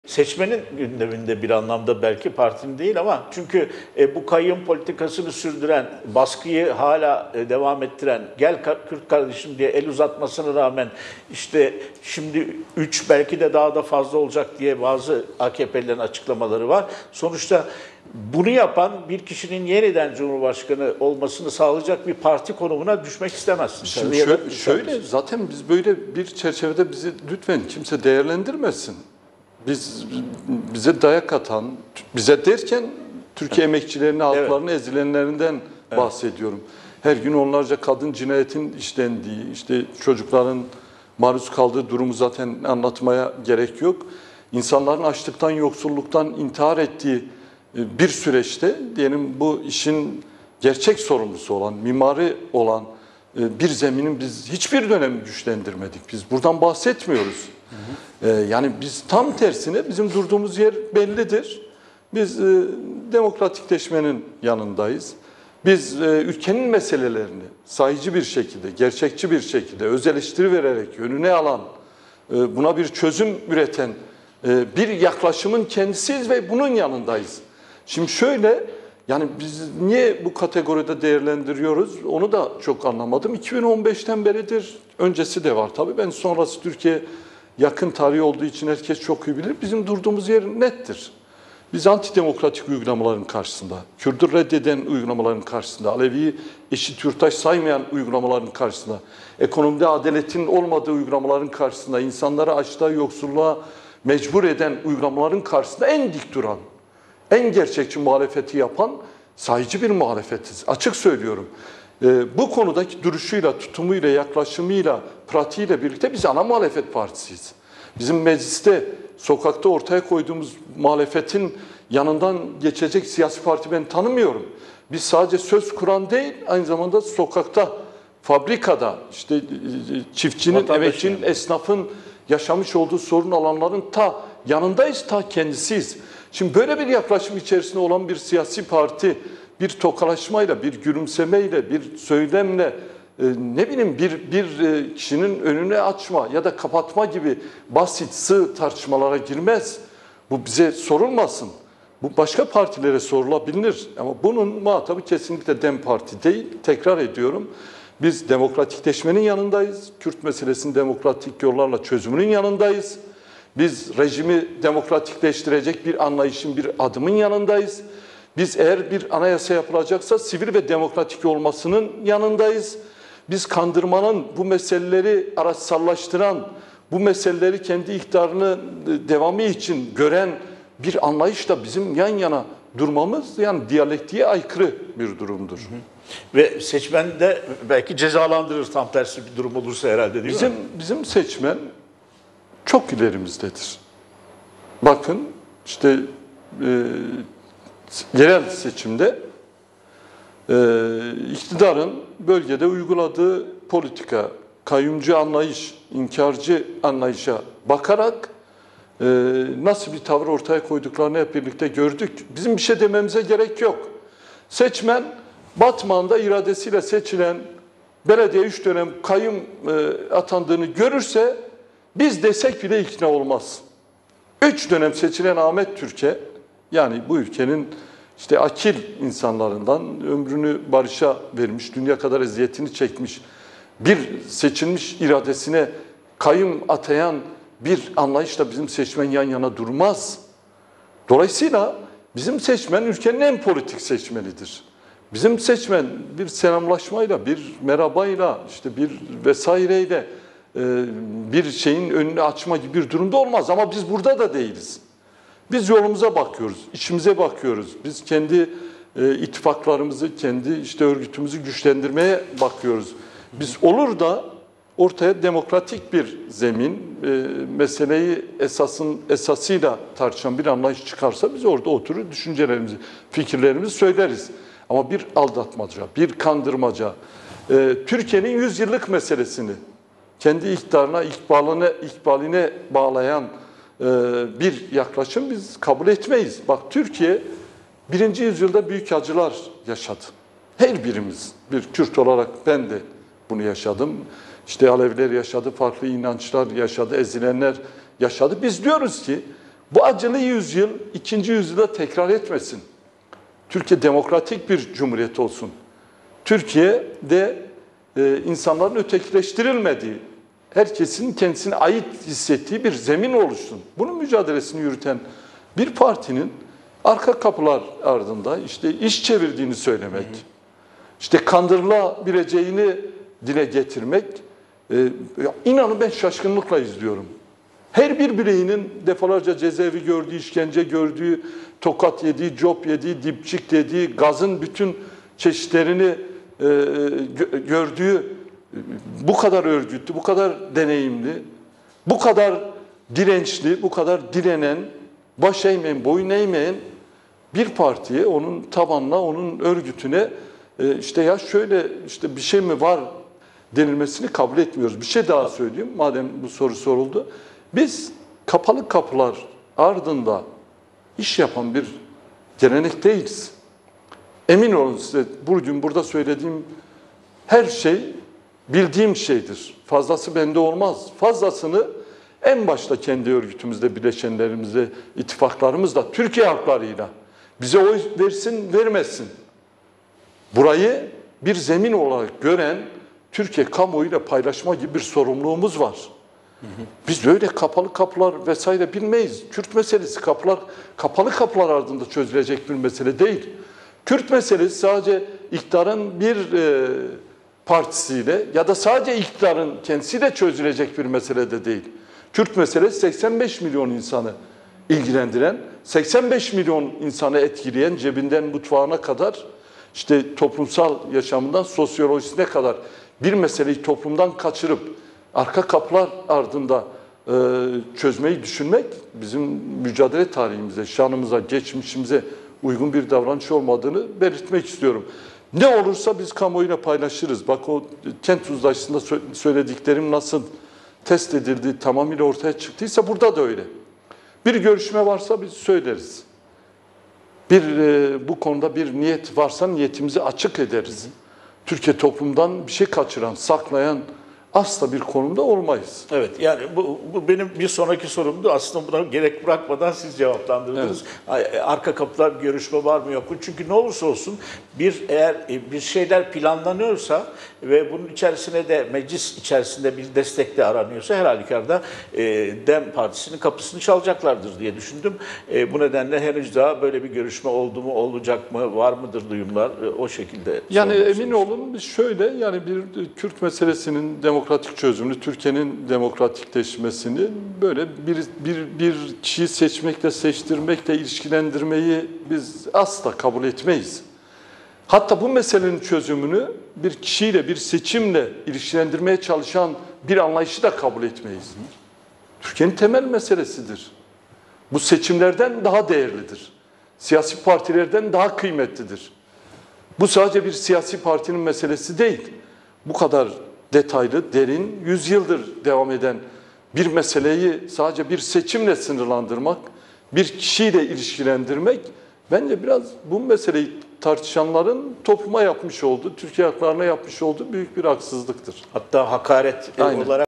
Kayyım Operasyonuna Tuncer Bakırhan'ın Tepkisi Ağır Oldu: Bir Selamlaşma Bir Gülümsemeyle...
DEM Parti Eş Genel Başkanı Tuncer Bakırhan Esenyurt, Mardin, Batman ve Halfeti belediyelerine kayyım atanmasına tepki gösterdi: Kayyımcı iradeyle seçmenimiz yan yana durmaz.